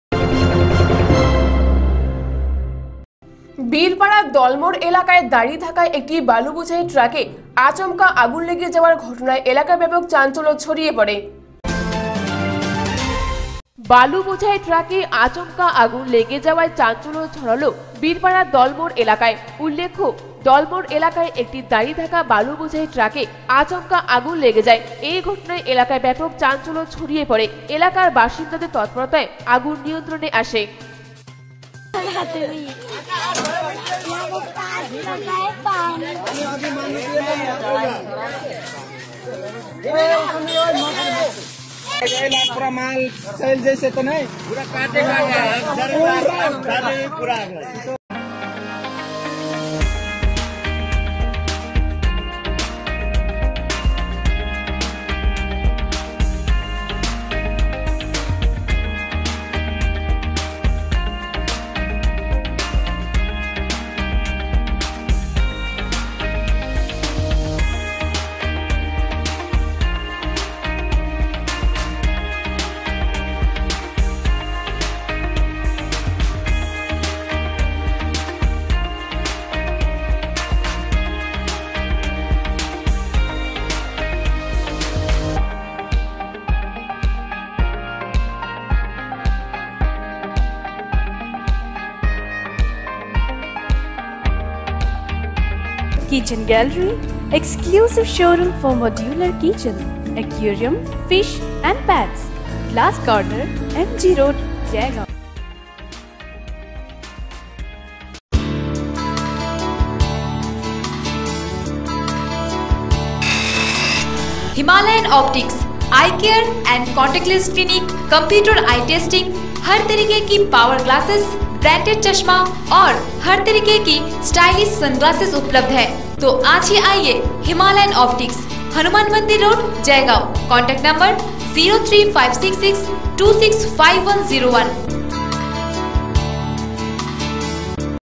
বীরপাড়া দলমোর এলাকায় দাড়িয়ে থাকা বালু বোঝাই ট্রাকে আচমকা আগুন লেগে যাওযায় এলাকায় ব্যাপক চাঞ্চল্য
বীরপাড়া দলমোর এলাকায় একটি দাড়িয়ে থাকা বালু বোঝাই ট্রাকে আচমকা আগুন লেগে যাওযায় এলাকায় ব্যাপক চাঞ্চল্য ছড়িয়ে পড়ে